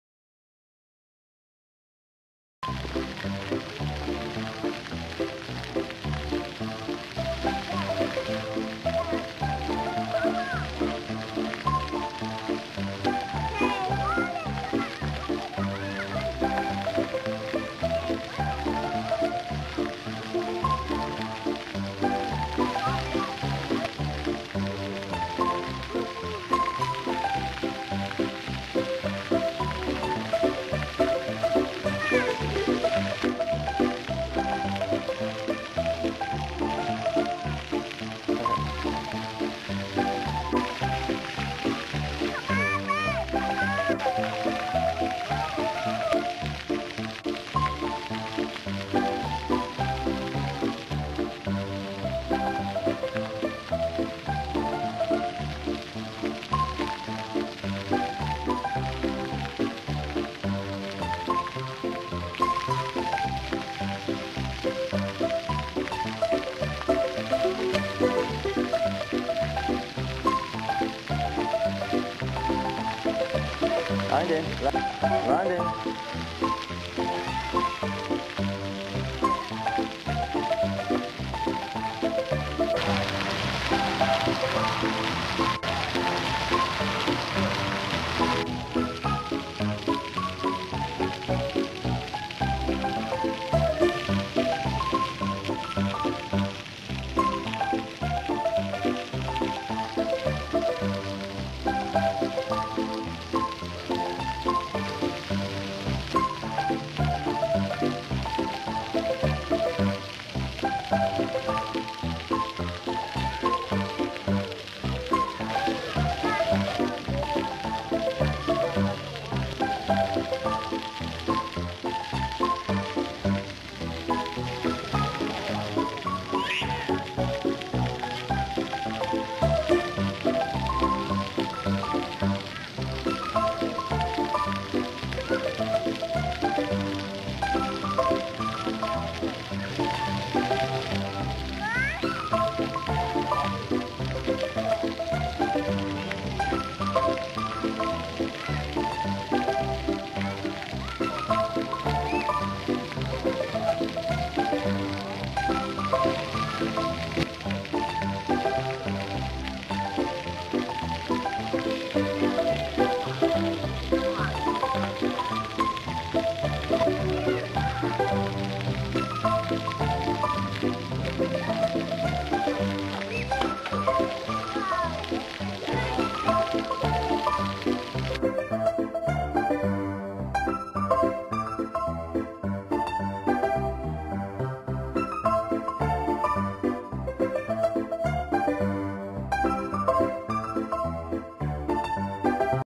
Old timey fun at the splash park.
Kids at the splash pad, at the park..and fun in the sun...